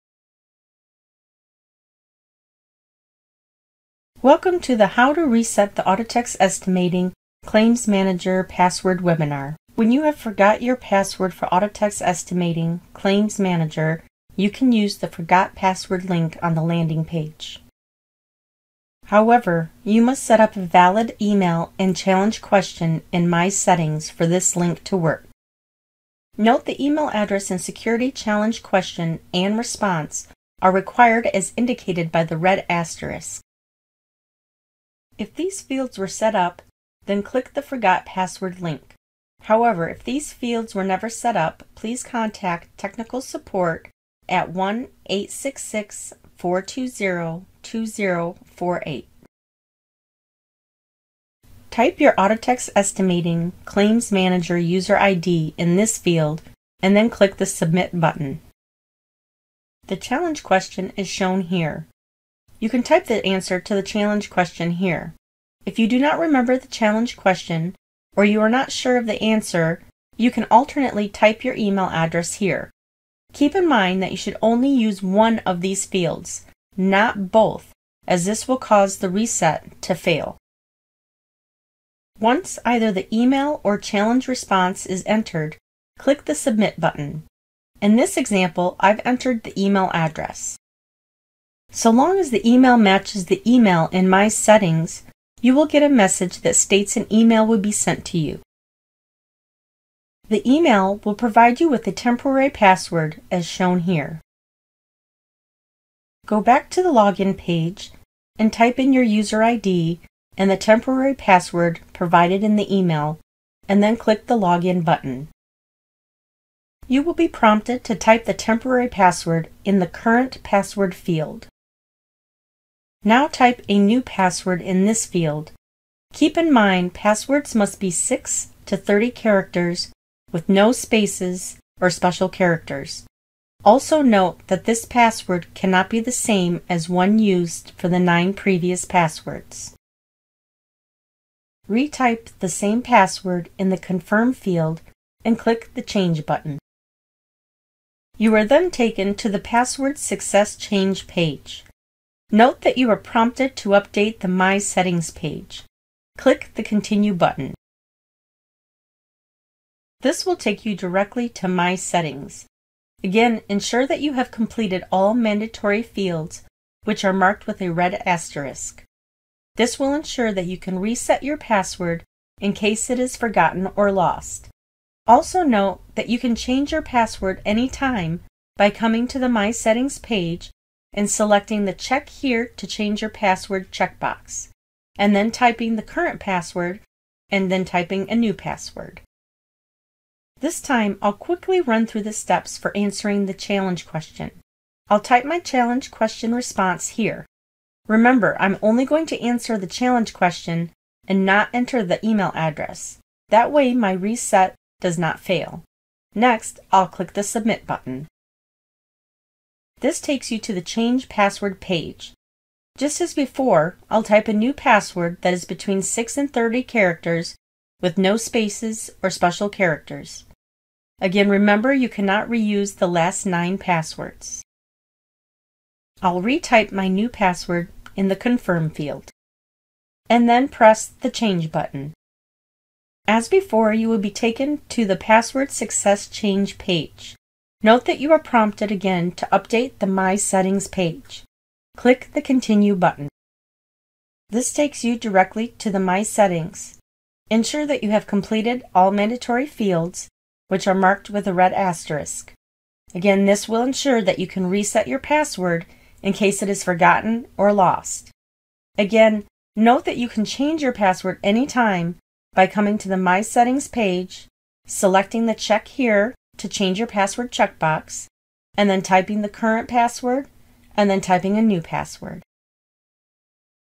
Reset Your Audatex Estimating or Claims Manager Password
Watch this video to learn how to reset your Audatex Estimating or Claims Manager password.